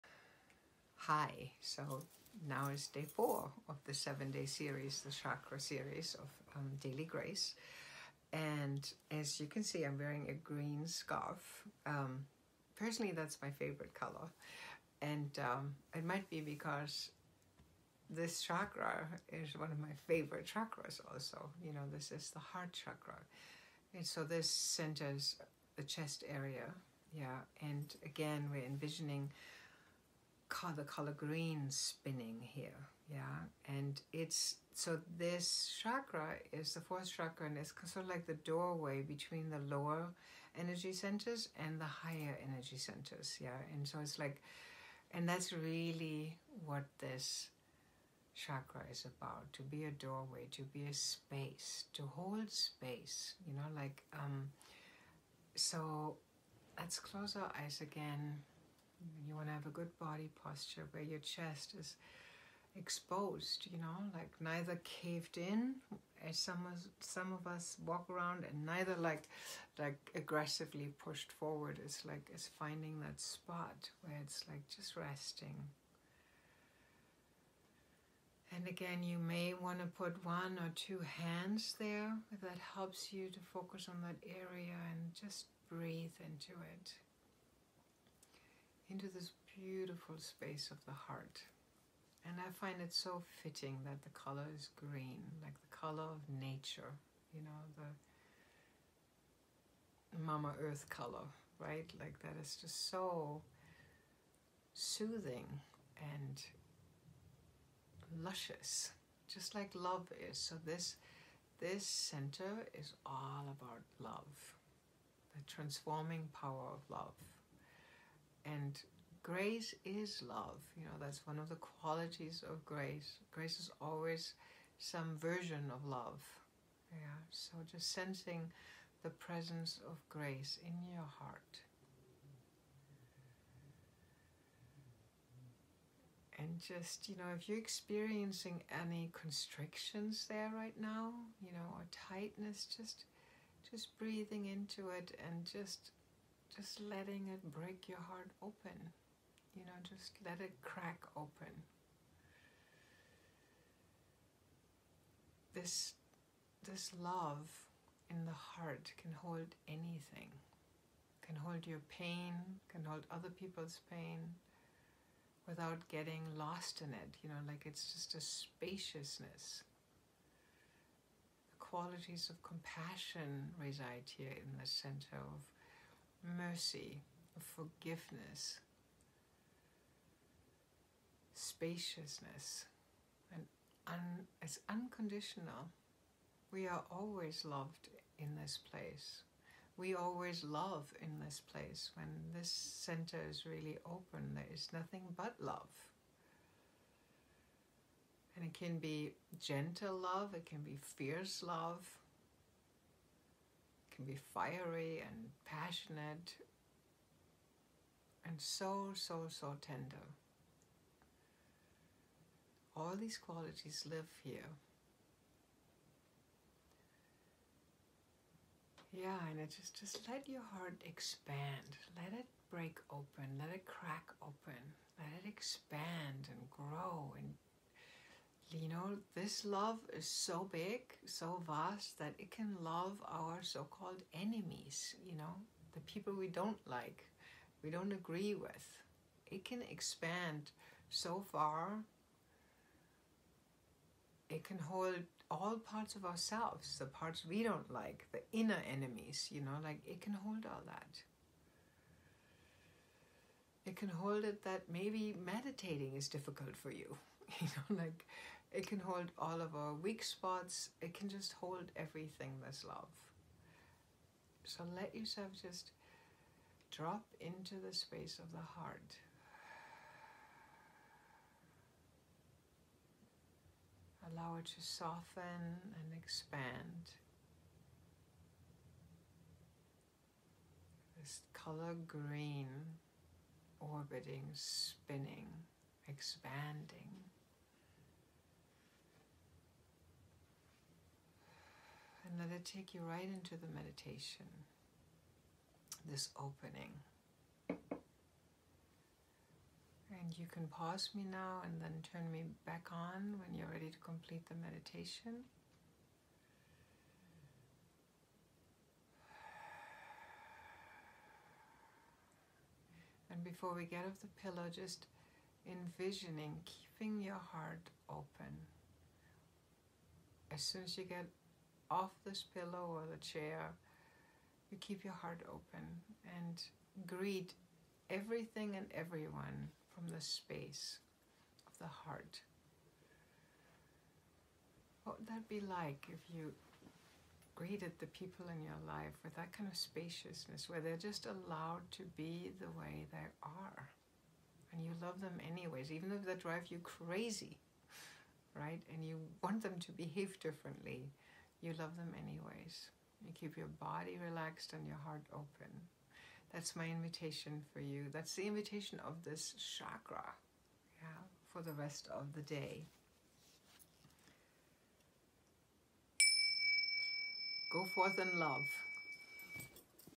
Chakra Meditation Series- 4th Chakra
4rd Chakra: Heart (Anahata) Join Britta Eskey in this 7-Day Chakra Meditation Series to assist you in receiving the Grace that uniquely resides in each of these beautiful energy centers.